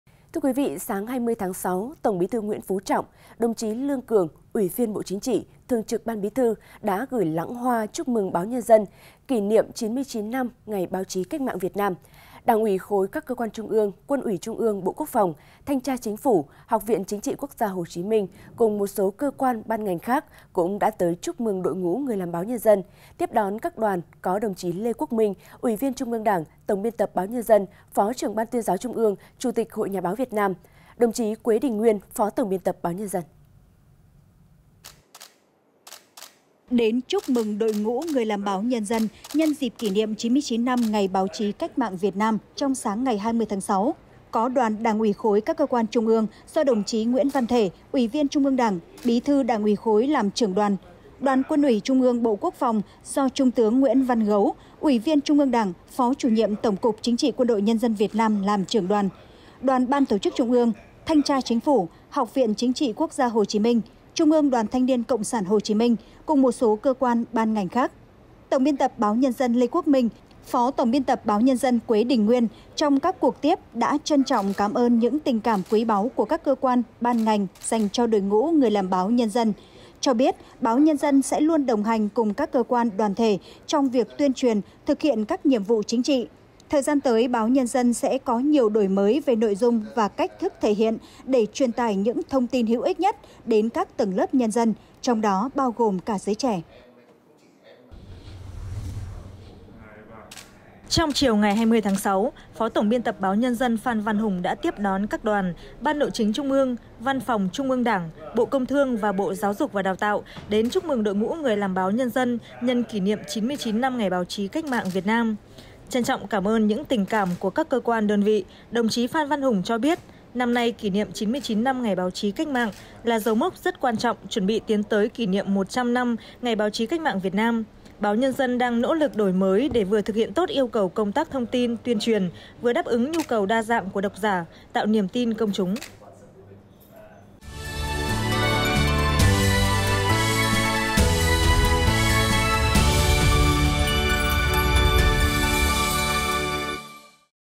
Các cơ quan chúc mừng Báo Nhân Dân nhân ngày 21/6 | Đảng với Dân
Sáng nay (20/6) – Tổng Bí Thư Nguyễn Phú Trọng, đồng chí Lương Cường, Ủy viên Bộ chính trị - Thường trực Ban Bí thư đã gửi lẵng hoa chúc mừng Báo Nhân  Dân - Nhân kỷ niệm 99 năm ngày báo chí CM.

Đảng ủy Khối các cơ quan Trung ương, Quân uỷ trung ương - Bộ Quốc phòng, Thanh Tra Chính Phủ, Học viện Chính trị quốc gia Hồ Chí Minh cùng một số cơ quan, ban ngành khác cũng đã tới chúc mừng Đội ngũ người làm báo Nhân Dân –Tiếp đón các đoàn có đồng chí Lê Quốc Minh, Ủy viên TW Đảng, Tổng Biên Tập báo Nhân Dân, Phó Trưởng Ban Tuyên giáo Trung ương,  Chủ tịch Hội Nhà báo Việt Nam. Đồng chí Quế Đình Nguyên, Phó tổng Biên tập Báo Nhân Dân.

Đến Chúc mừng đội ngũ người làm báo Nhân dân dịp kỷ niệm 99 năm ngày báo chí Cách mạng Việt Nam trong sáng 20/6 có Đoàn Đảng ủy Khối các cơ quan Trung ương do Đồng chí Nguyễn Văn Thể, ủy viên TW Đảng, Bí thư Đảng ủy Khối làm trưởng đoàn; Đoàn Quân ủy trung ương - Bộ Quốc phòng do  Trung tướng Nguyễn Văn Gấu, Uỷ viên Trung ương Đảng, Phó Chủ nhiệm Tổng cục Chính trị QĐND Việt Nam làm trưởng đoàn, Đoàn Ban tổ chức Trung ương, Thanh Tra Chính Phủ; Học viện Chính trị quốc gia Hồ Chí Minh, Trung ương Đoàn Thanh niên Cộng sản Hồ Chí minh cùng một số cơ quan, ban ngành khác ….

Tổng Biên tập Báo Nhân Dân Lê Quốc Minh, Phó Tổng Biên tập Báo Nhân Quế Đình Nguyên trong các cuộc tiếp đã trân trọng cảm ơn những tình cảm quý báu của các Cơ quan, ban ngành dành cho đội ngũ người làm báo Nhân Dân, cho biết: báo Nhân Dân sẽ luôn đồng hành cùng các cơ quan, đoàn thể trong việc tuyên truyền, thực hiện các nhiệm vụ chính trị. Thời gian tới báo Nhân Dân sẽ có nhiều đổi mới về nội dung và cách thức thể hiện, để truyền tải những thông tin hữu ích nhất đến các tầng lớp nhân dân trong đó bao gồm cả giới trẻ.